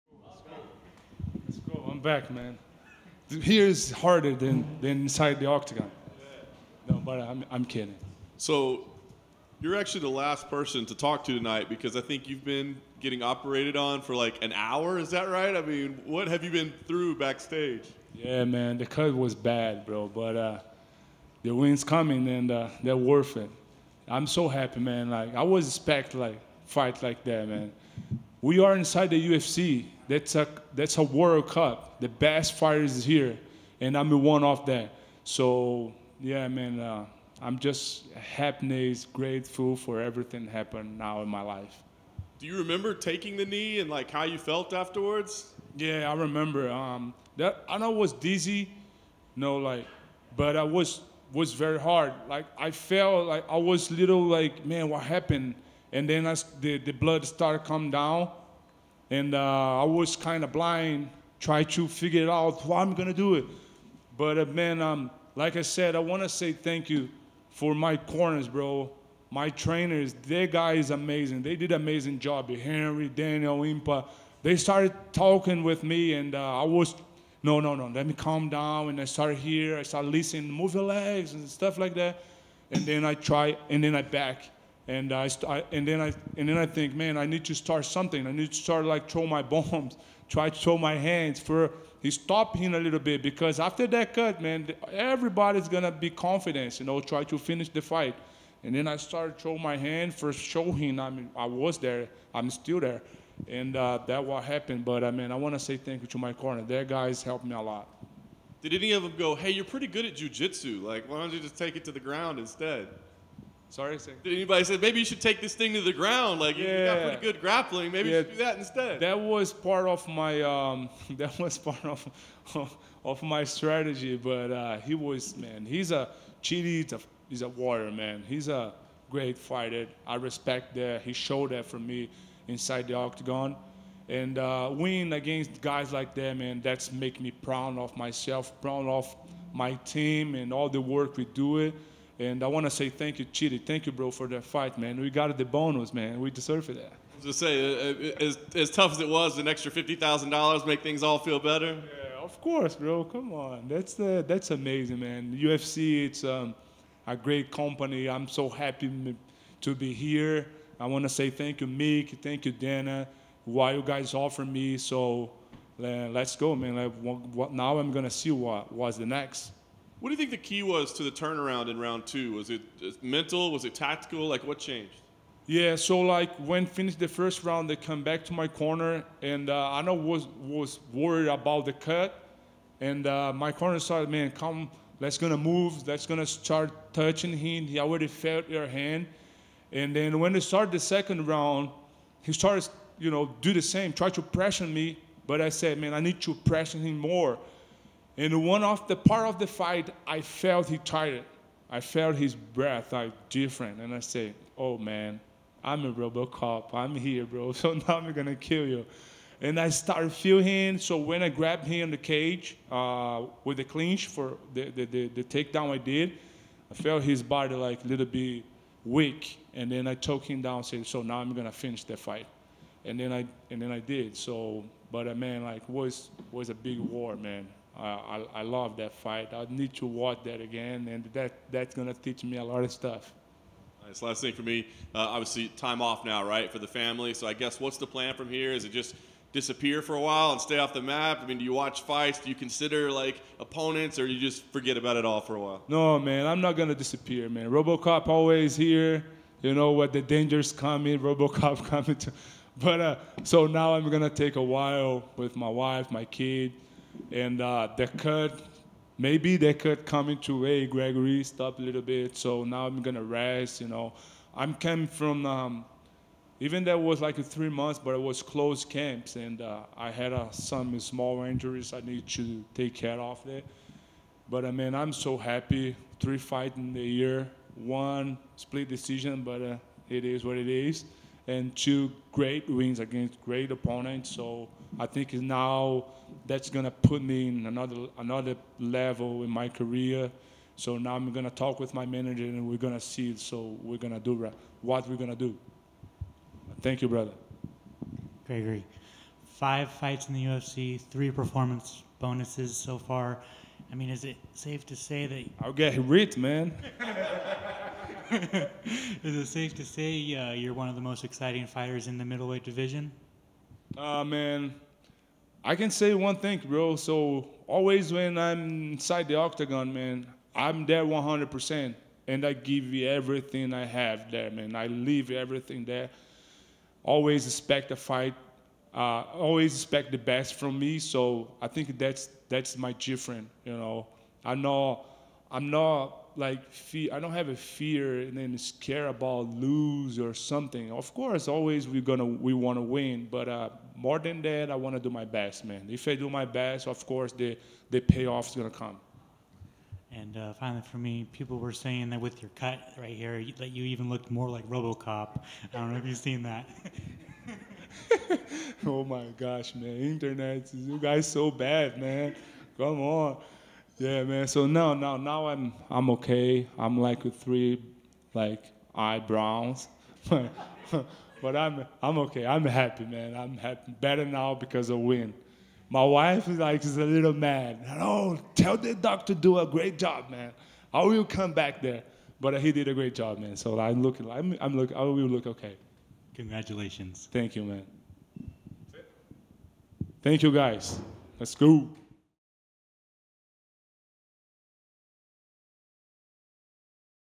Gregory Rodrigues On Horrific Cut Before TKO: 'I Have 3 Eyebrows' | UFC Fight Night 210
Gregory Rodrigues talks to MMA Junkie at post-fight at UFC Fight Night 210 media day after his second-round TKO of Chidi Njokuani on Saturday in Las Vegas.